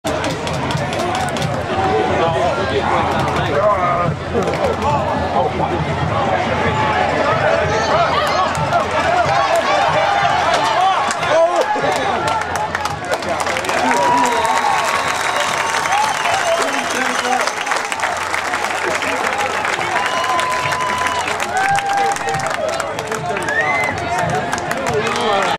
Andrew Cole on the punt return.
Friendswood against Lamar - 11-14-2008